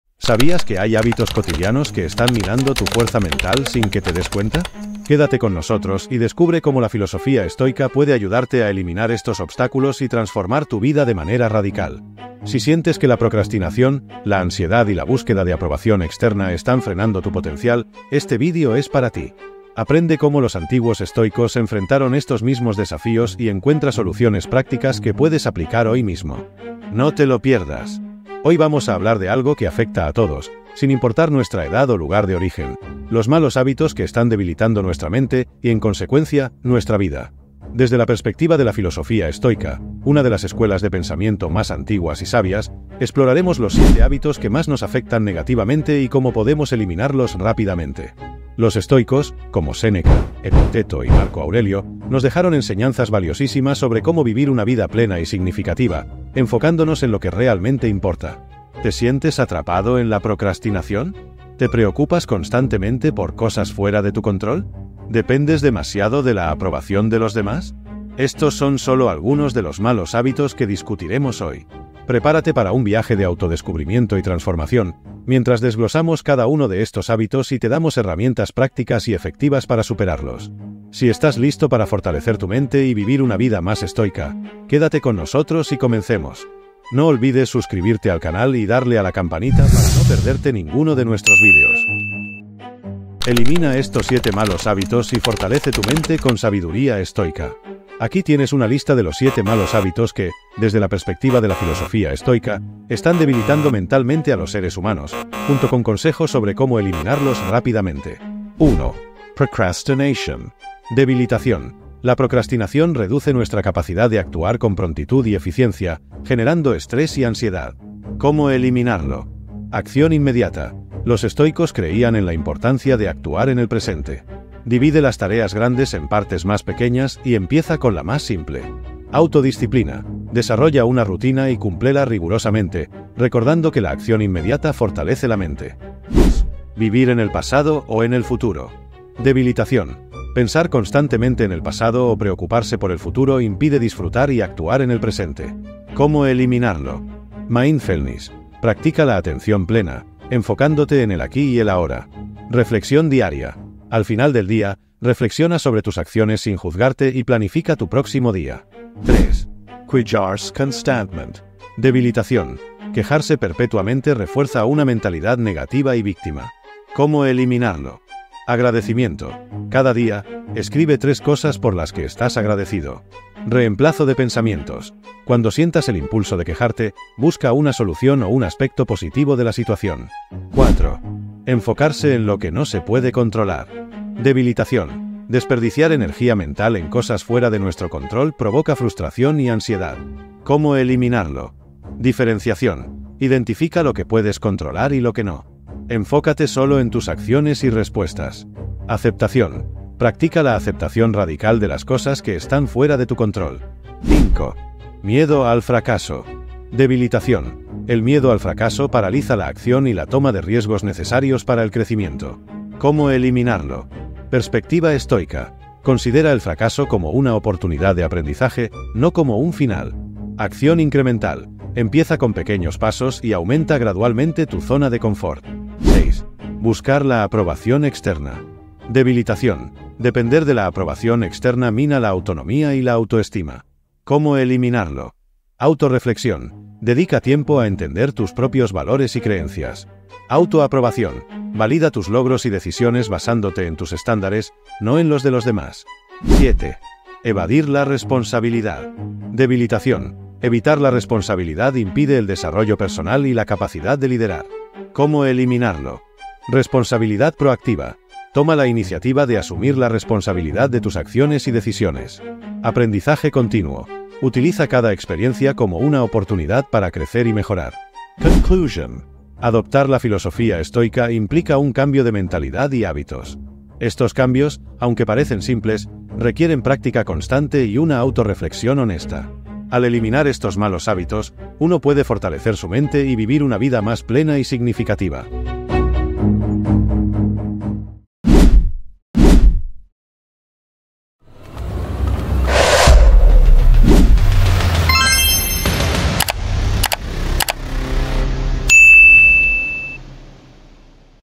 🎙️🧠 "Elimina Estos 7 Malos Hábitos y Fortalece tu Mente con Sabiduría Estoica"
¡Descubre cómo la filosofía estoica puede transformar tu vida! En este video, exploramos los 7 malos hábitos que están debilitando tu mente y cómo eliminarlos rápidamente. Aprende de los grandes pensadores estoicos como Séneca, Epicteto y Marco Aurelio, y encuentra soluciones prácticas para superar la procrastinación, la ansiedad, la búsqueda de aprobación externa y más.

Este video es una guía esencial para fortalecer tu mente y vivir una vida más plena y significativa. No te pierdas estos valiosos consejos que te ayudarán a enfrentar tus desafíos diarios con una nueva perspectiva.

✨ Temas que cubrimos:
✅ Cómo la procrastinación afecta tu vida y cómo combatirla
✅ La importancia de vivir en el presente según los estoicos
✅ Estrategias para dejar de quejarte y adoptar una mentalidad positiva
✅ Enfocarse en lo que puedes controlar y aceptar lo demás
✅ Superar el miedo al fracaso con sabiduría estoica
✅ Dejar de depender de la aprobación externa y encontrar tu valor interno
✅ Asumir la responsabilidad y crecer personalmente